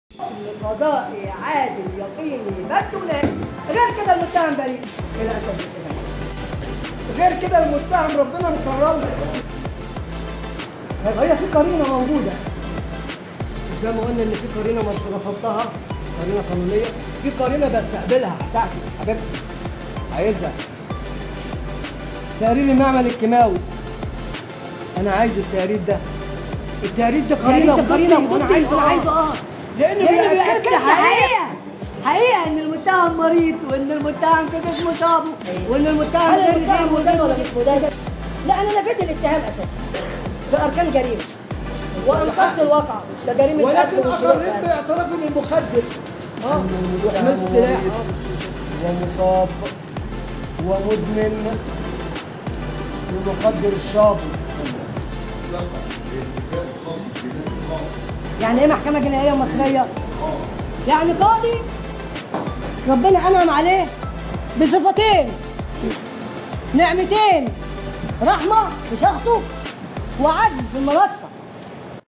جلسة الحكم على سفاح الاسماعيليه
جلسة الحكم على سفاح الاسماعيليه بتمثيل الجريمه بحضور النيابة العامه
ويوكد الدفاع ان القاتل كان يتعاطا موخدر الشابو